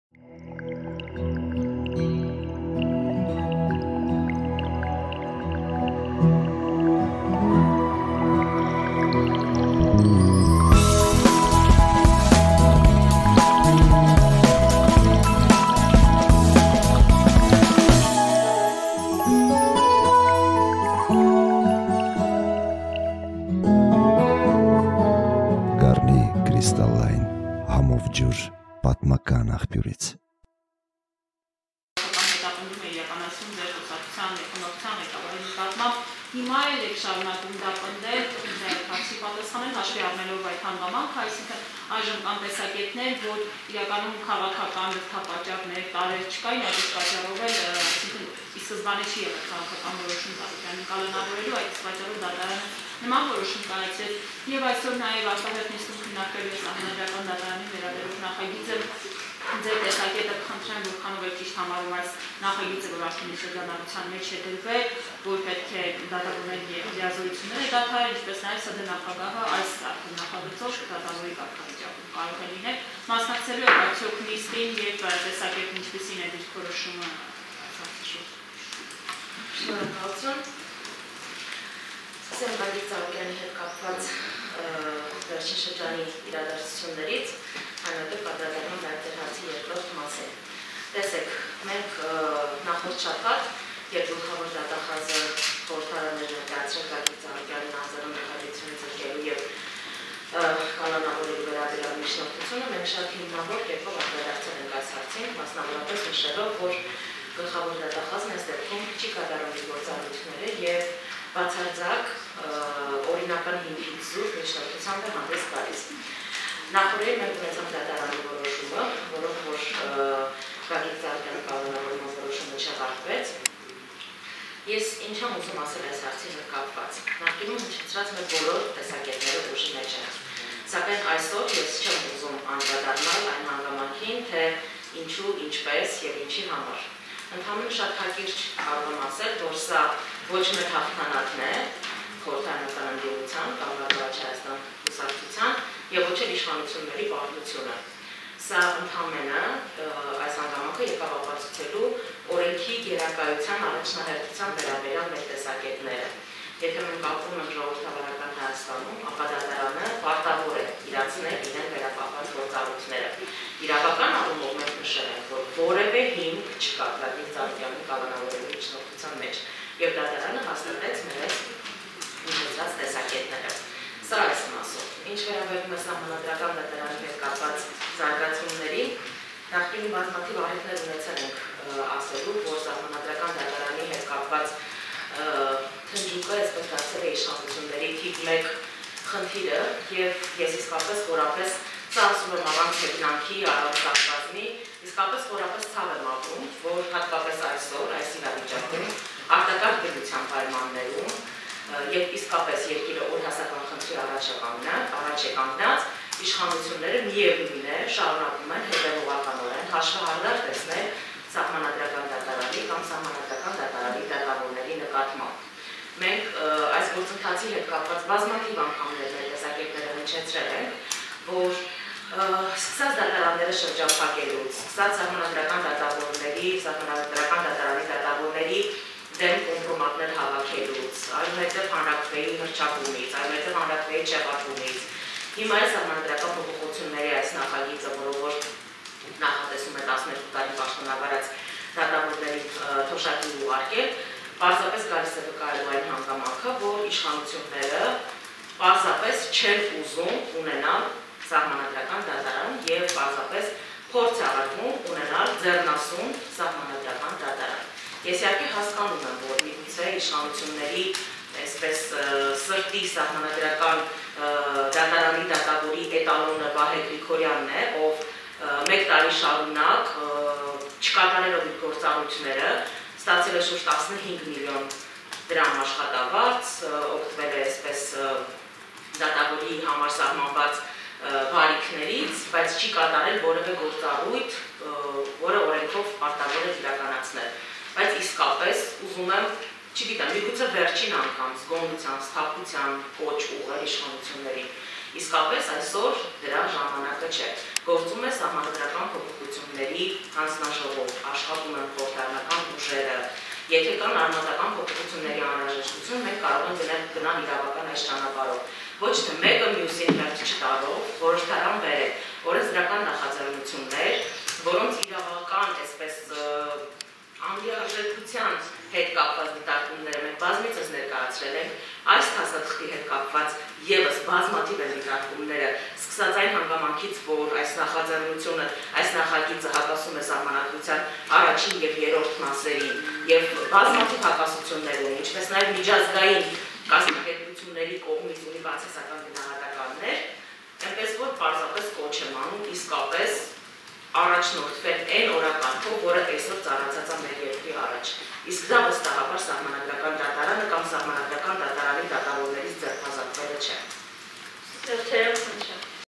Իշխանության սրտի ՍԴ դատավորը 15 մլն դրամ աշխատավարձ ստացող ու ոչինչ չանող Վահե Գրիգորյանն է.Տոնոյան
Հետևե՛ք «Հայելի ակումբին»